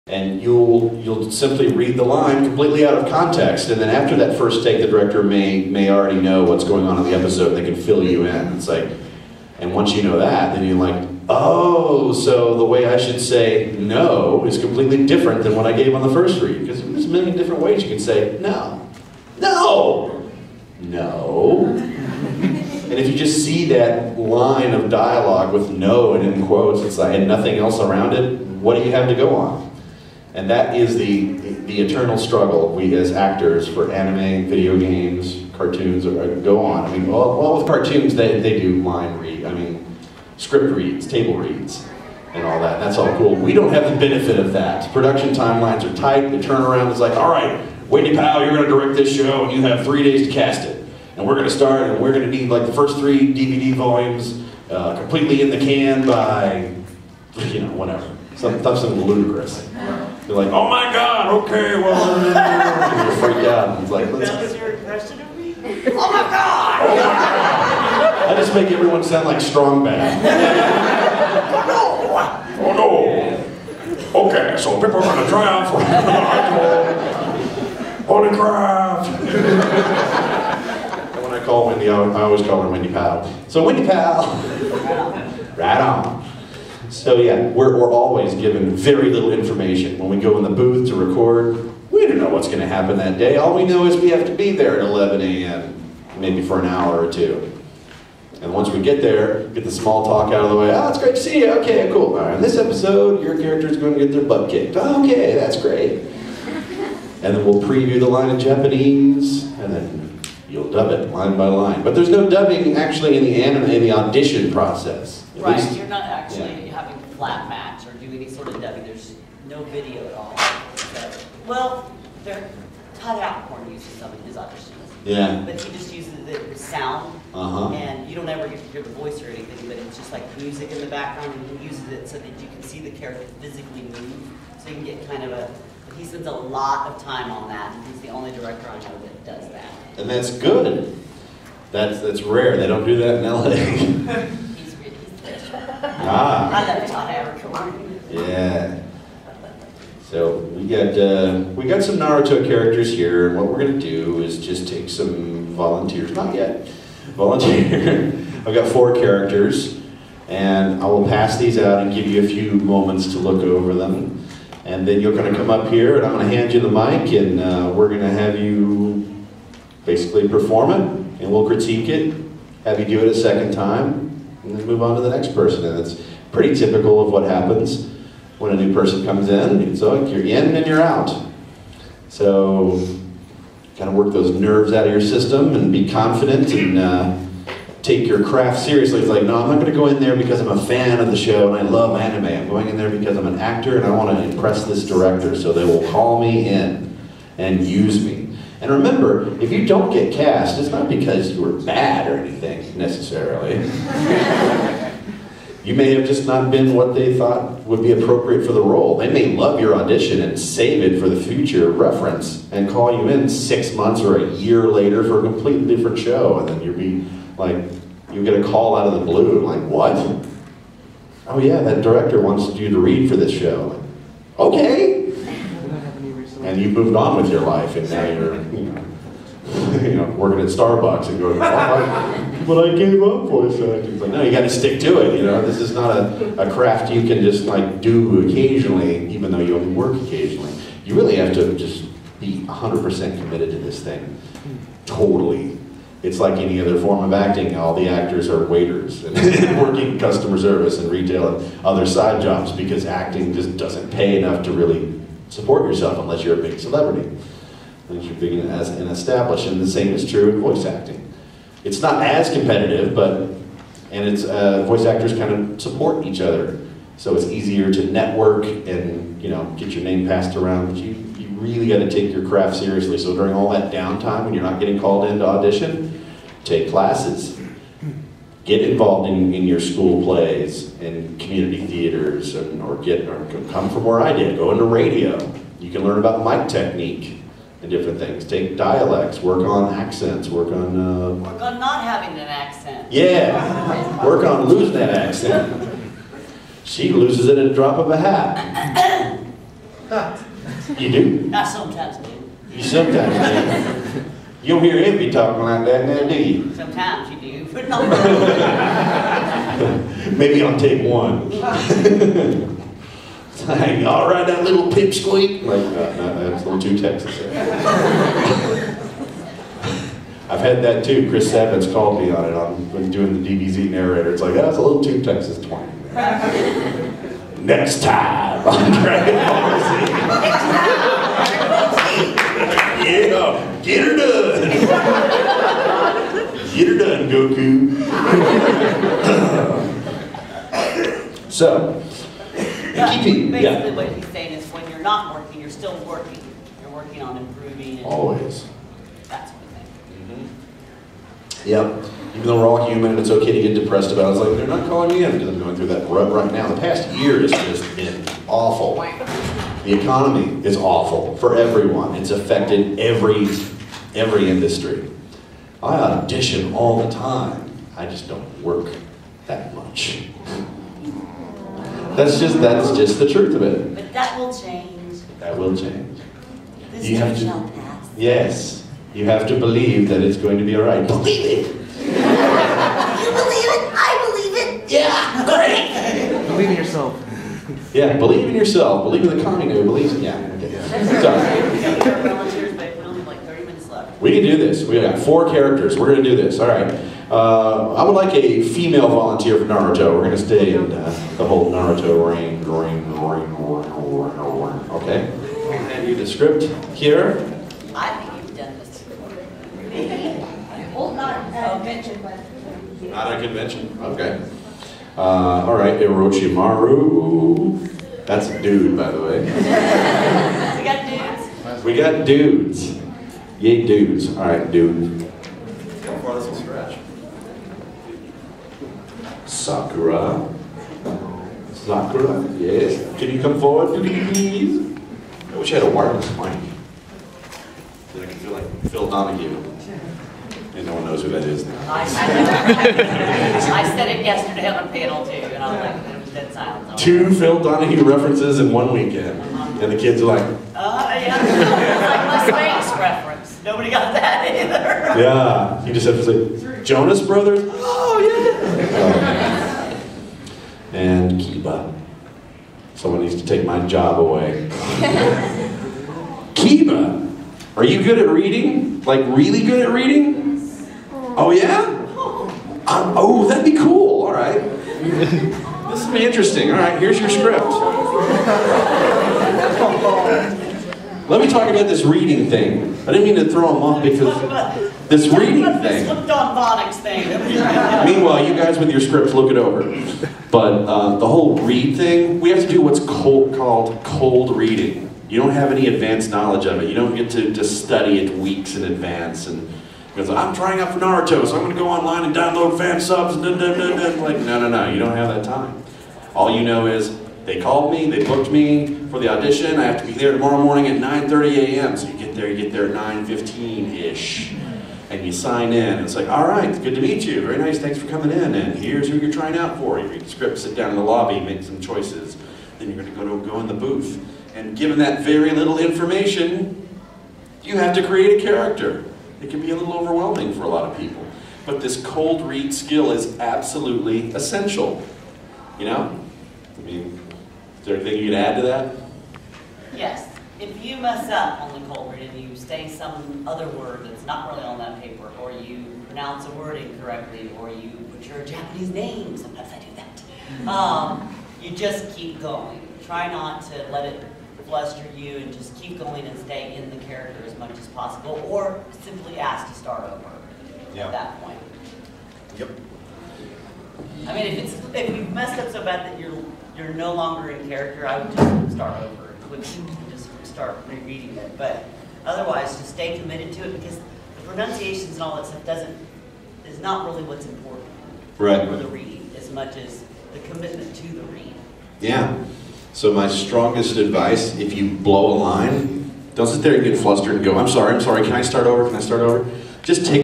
Anime Audition Panel Pt.2
Anime Audition Panel at San Japan 2.X 2009. I had to break the video into 5 parts for YouTube, each video overlaps by about 20 seconds so that you wont miss anything in the transitions.